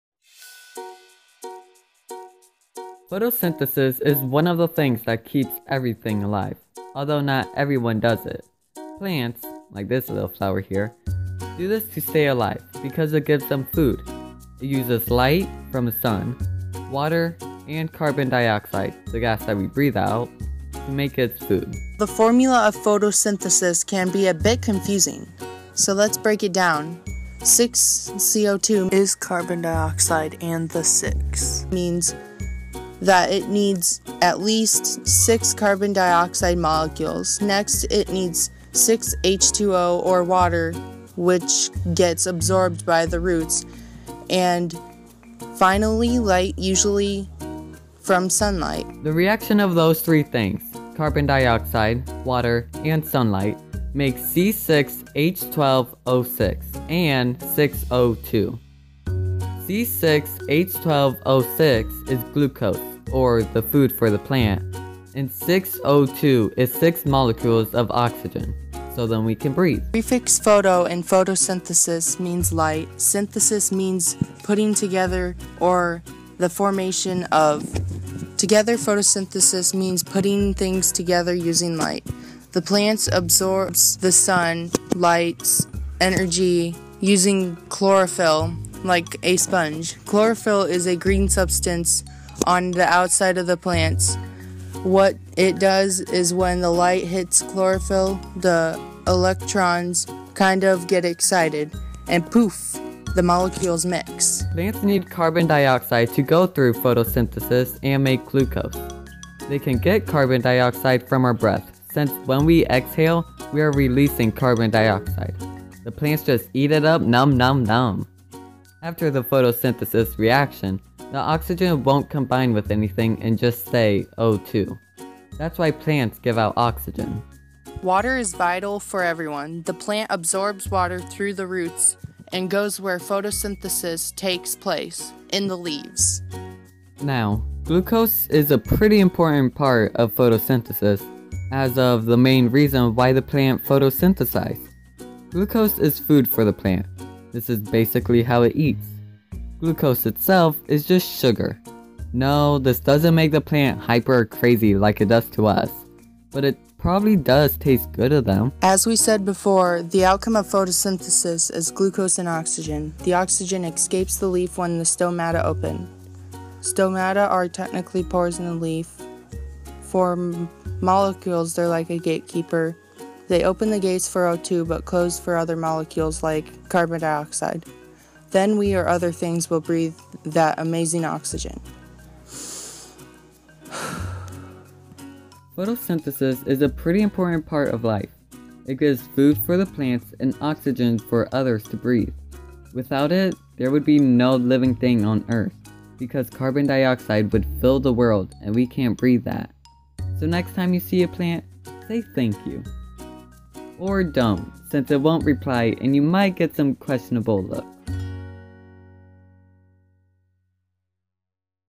Photosynthesis Explaination
This video is meant for a biology project but if you need help with photosynthesis then be my guest and watch it!